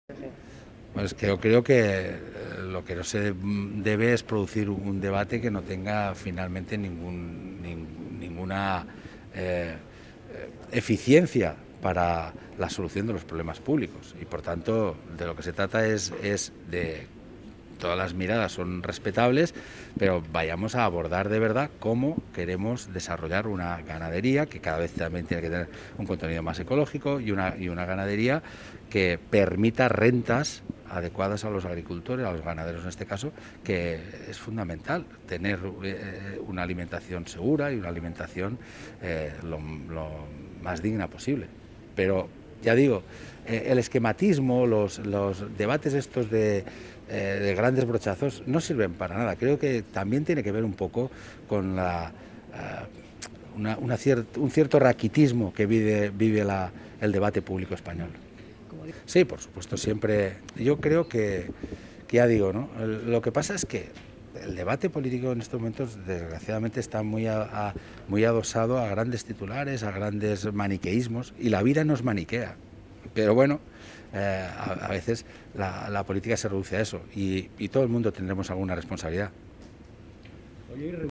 Puig ve "cierto raquitismo" en el debate sobre las macrogranjas
El presidente de la Generalitat Valenciana, Ximo Puig, ha rechazado generar polémicas "estériles" sobre la ganadería y ha abogado por abordar, "de verdad y desde la seriedad", las reformas que se tengan que producir. Así lo ha manifestado al ser preguntado por las declaraciones del ministro de Consumo, Alberto Garzón, sobre las macrogranjas y cree que la polémica suscitada tiene que ver "con un cierto raquitismo que vive el debate público español".
(Fuente: Generalitat)